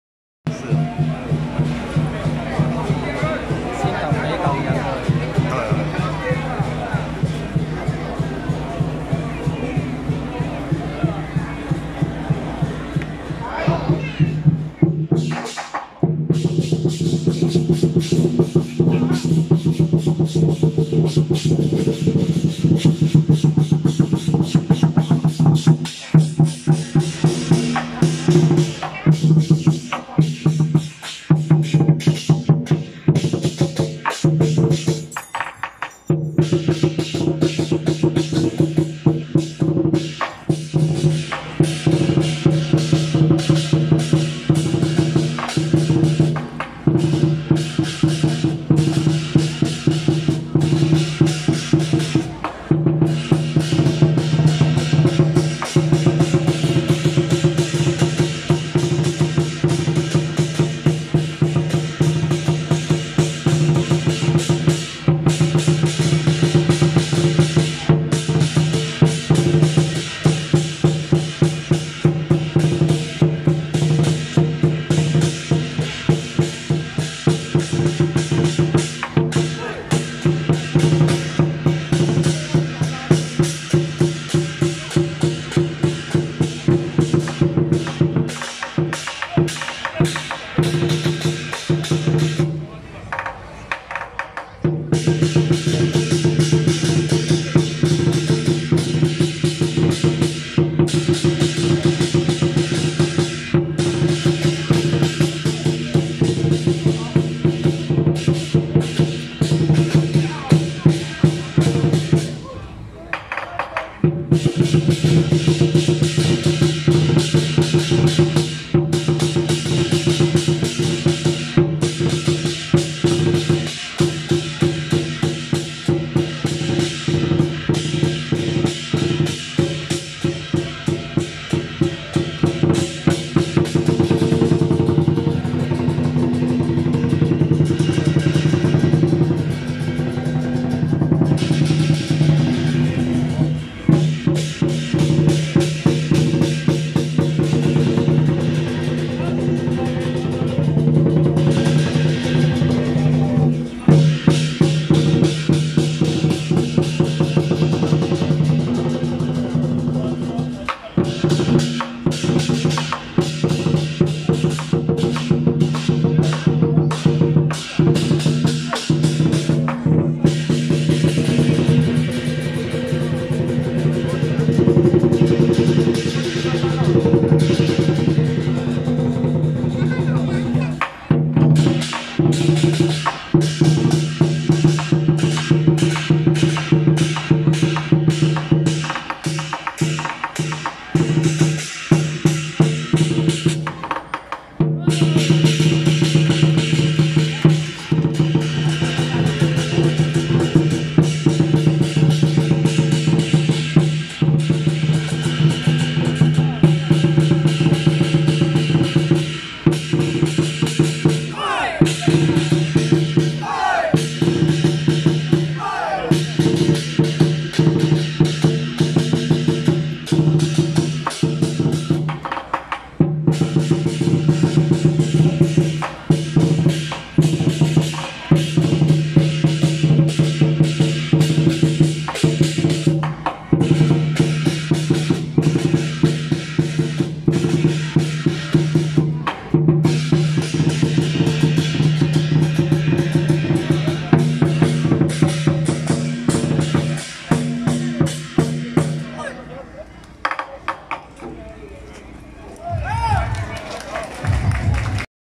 【新店聖和舘-醒獅團】~110景美景興太子宮天上聖母南巡謁祖進香回駕遶境
新店聖和舘-醒獅團